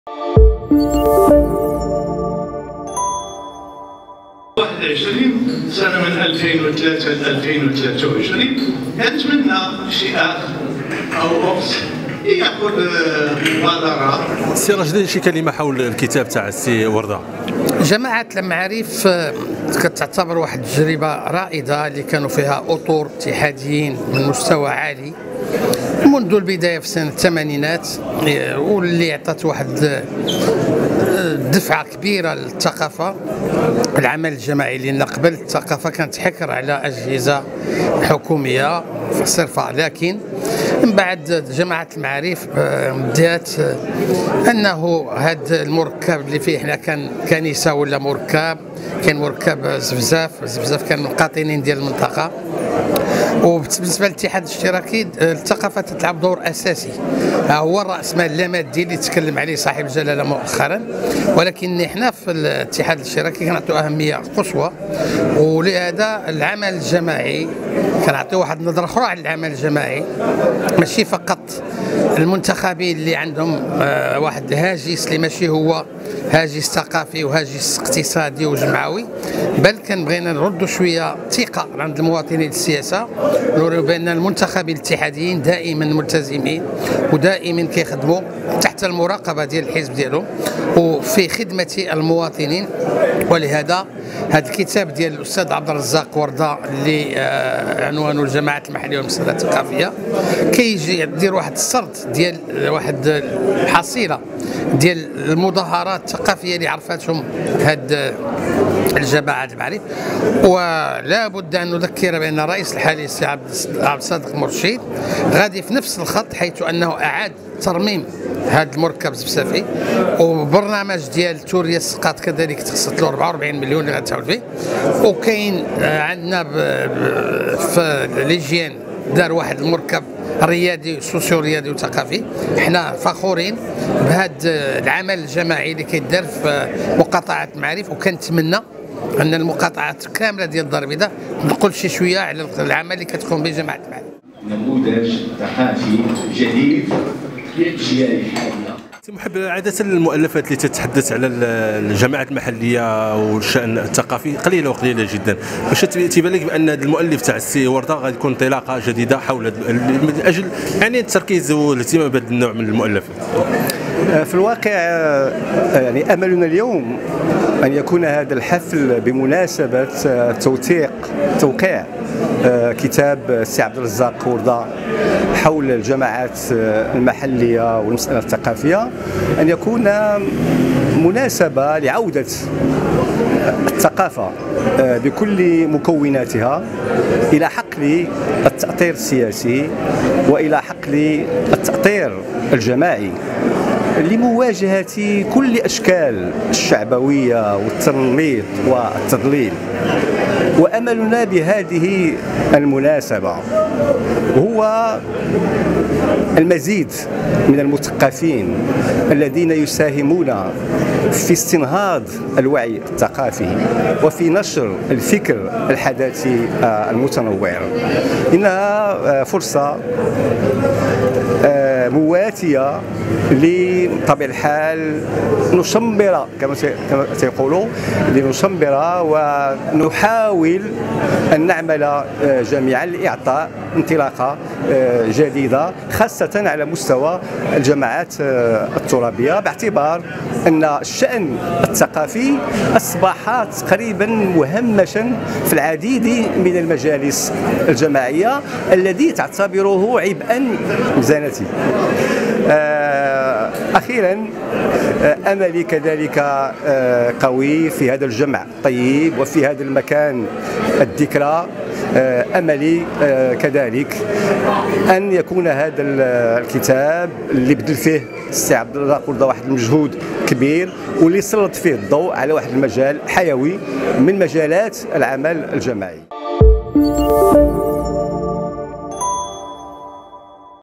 كتاب "الجماعات المحلية والمسألة الثقافية" تحت مجهر الراشدي ومحب
في هذا الفيديو يقدم محمد محب، الرئيس الأسبق لجماعة المعاريف بالبيضاء وإبراهيم الراشدي عضو المكتب السياسي لحزب الاتحاد الاشتراكي،  قراءة في كتاب "الجماعات المحلية والمسألة الثقافية " لصاحبه عبد الرزاق وردة